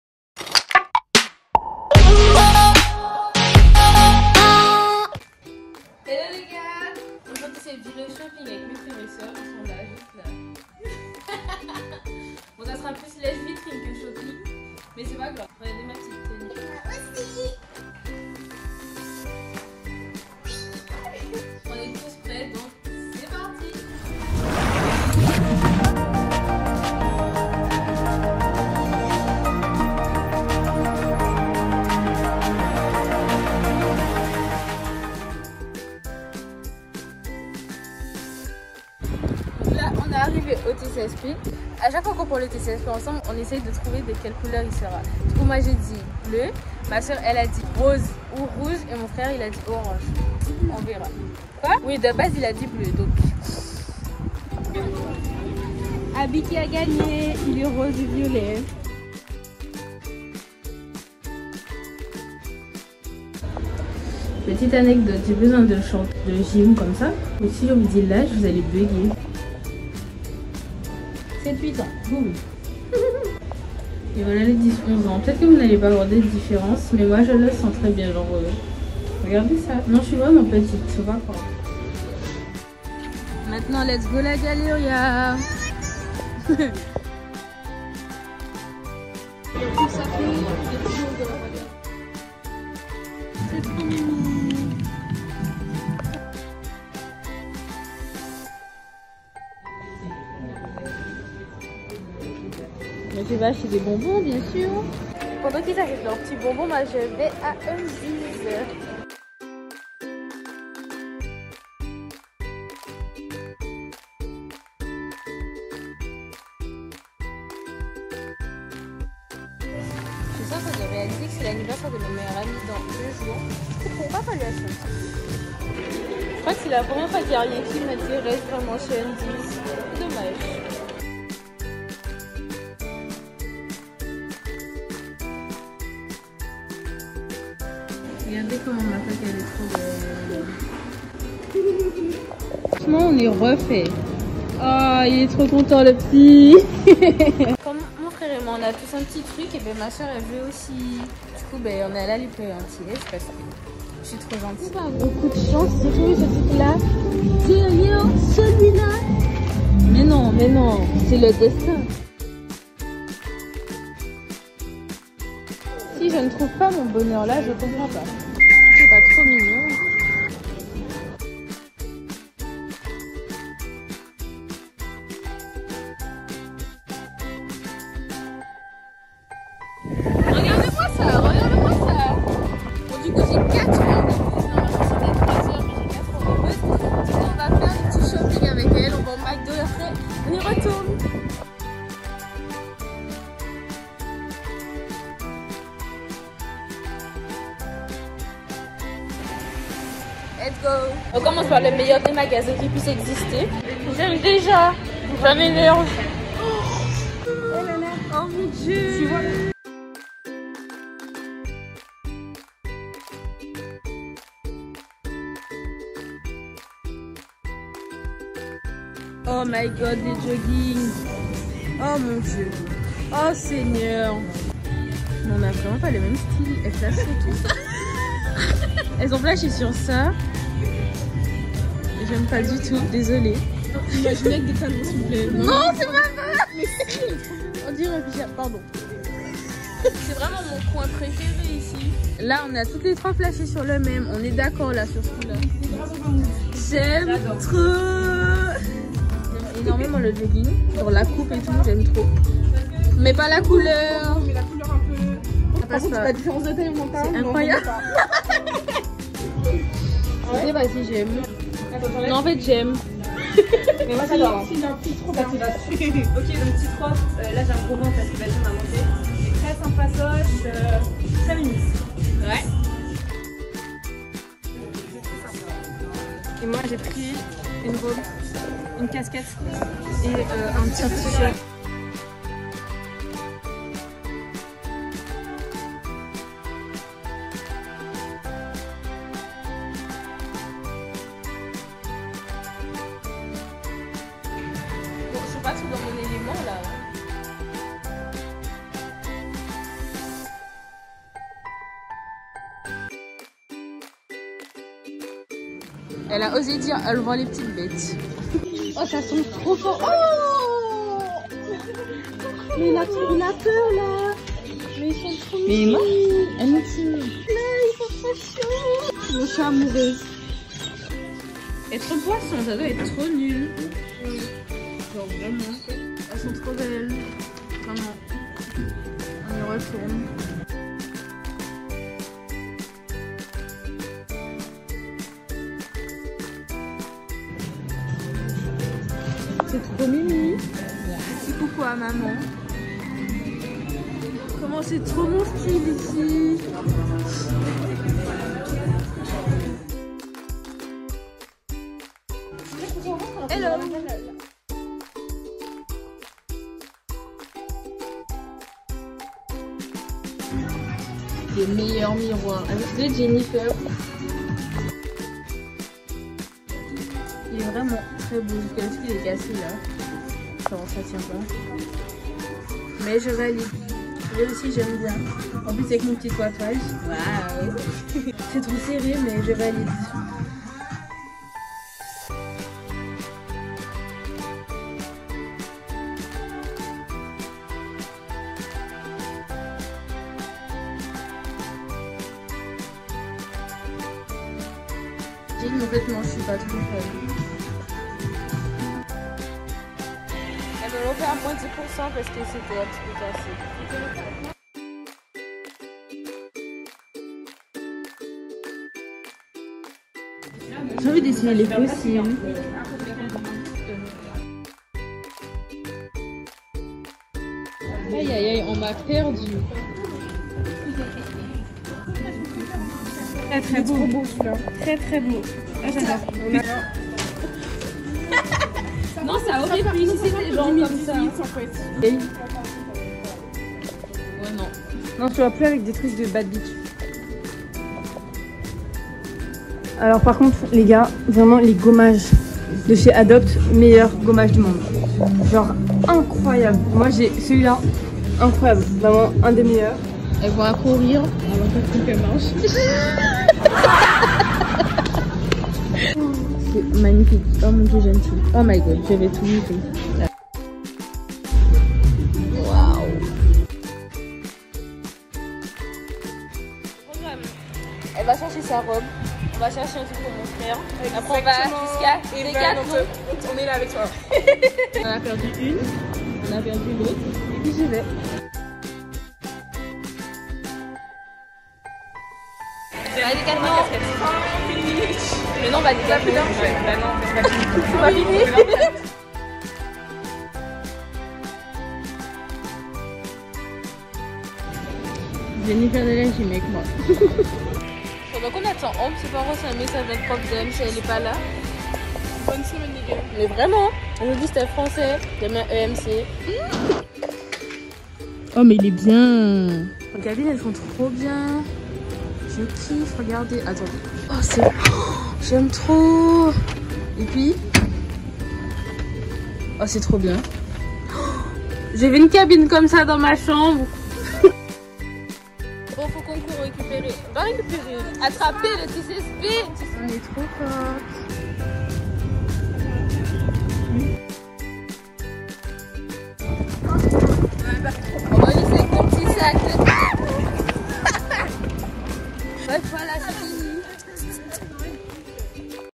JOURNÉE SHOPPING EN FAMILLE ET ENTRE AMIES 🛍
Une journée qui s’est transformé en 2 jours.
Pleins de magasins sympa à la Galléria ✨
Mon insta : @lana.nass_
Mon snap : @lana.pstl